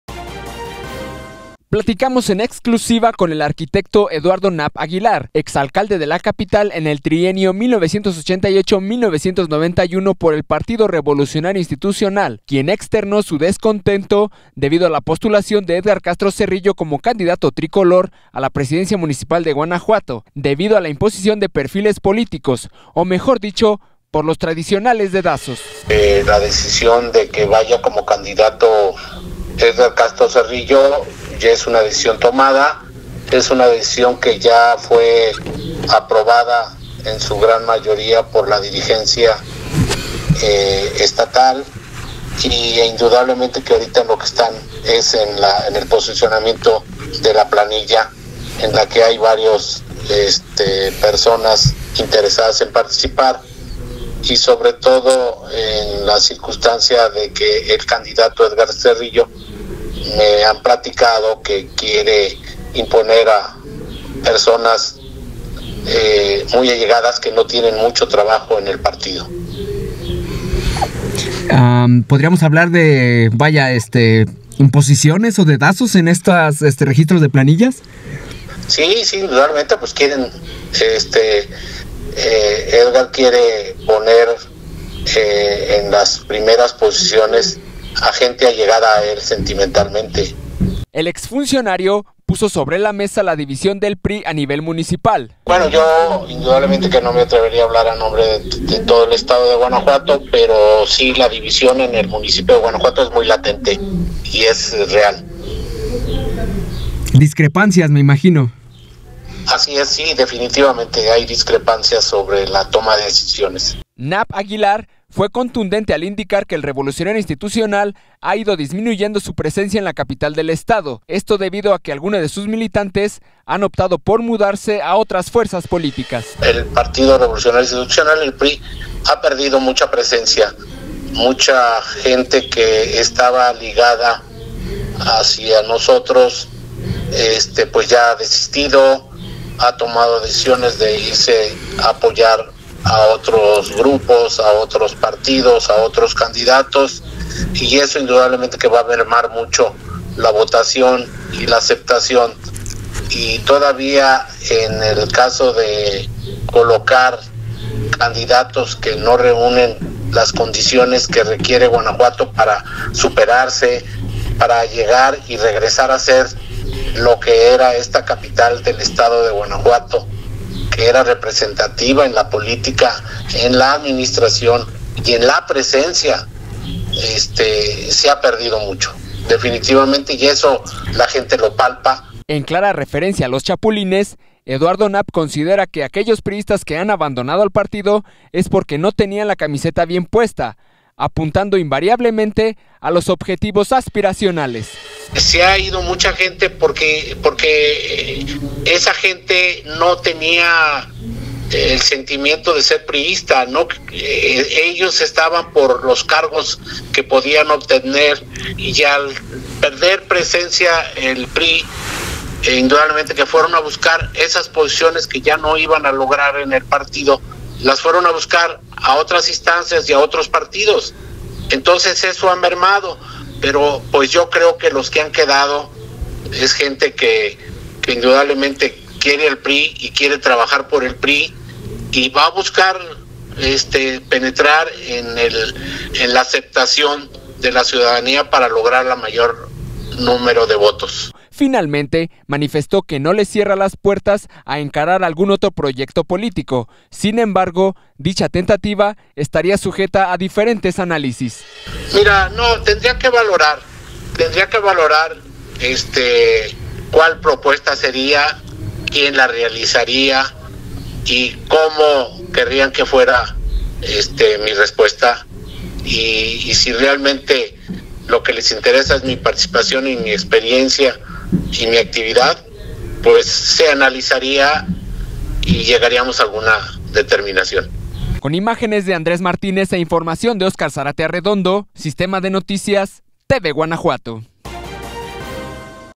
PriismoGuanajuatense, pues el ex-alcalde capitalino Eduardo Knapp reitera su descontento 😒
No todo es miel sobre hojuelas✨ para el PriismoGuanajuatense, pues el ex-alcalde capitalino Eduardo Knapp reitera su descontento 😒 por la candidatura de Edgar Castro Cerrillo a la presidencia municipal de Guanajuato